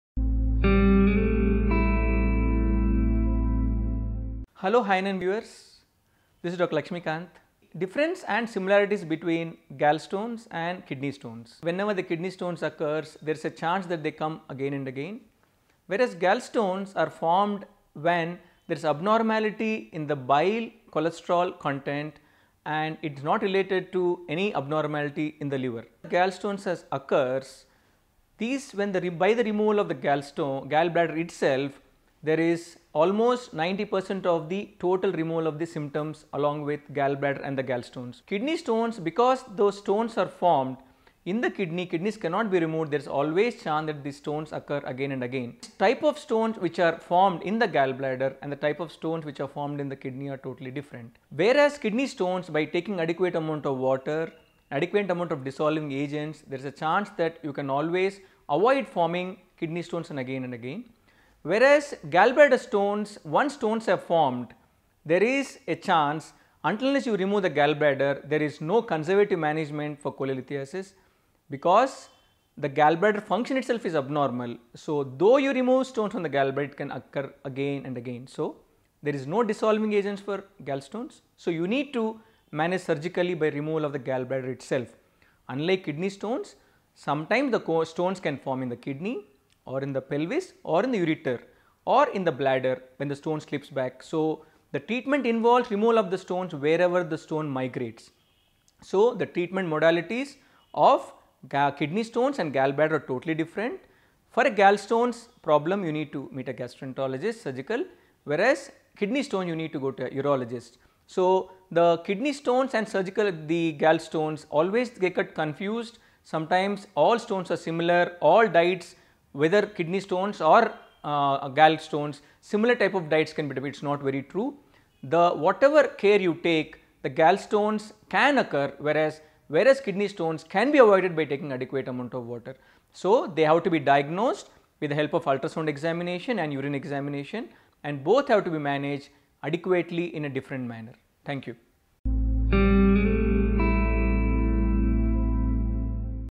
Hi9 | Gallstone vs Kidney Stone By Dr.T. Lakshmi kanth ,Surgical gasterontologist
Gallstone vs  Kidney Stone from Hi9 By Dr.T. Lakshmi kanth ,Surgical gasterontologist Star hospitals

A kidney stone is a solid mass made up of tiny crystals from the minerals calcium, oxalate and uric acid. Gallstones are hard, pebble-like cholesterol or pigment deposits that form inside the gallbladder. They can be as small as a grain of sand or as large as a golf ball.

hi9 hi9health hi9webtv gasterontology stones kidneystones gallstones bladderstones Gallstones vs. Kidney Stones